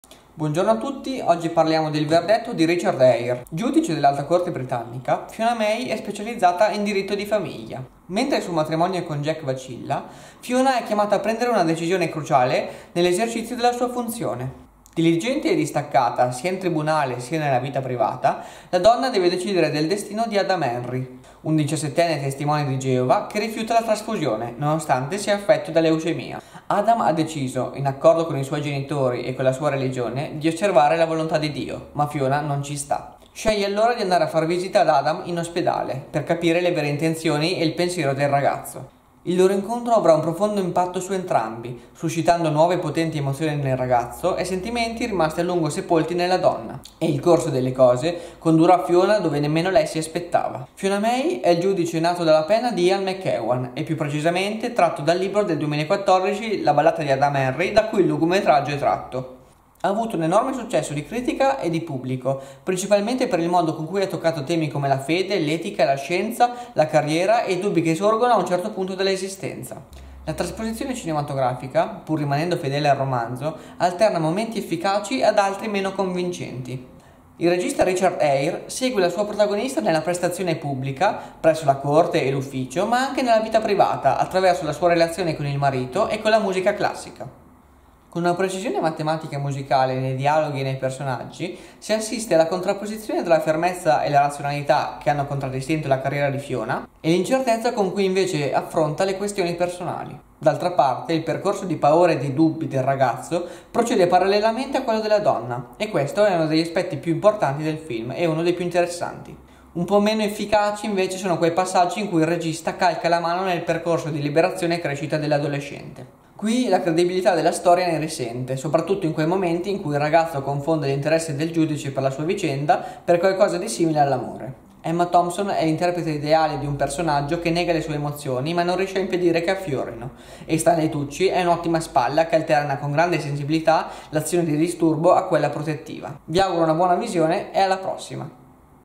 Succo d'Arancia - Il Verdetto [RECENSIONE FILM]
Quale sarà il verdetto su Il Verdetto di Simone?

Succo d'Arancia è una rubrica di recensioni cinematografiche ideata e realizzata da Simone Schiavi.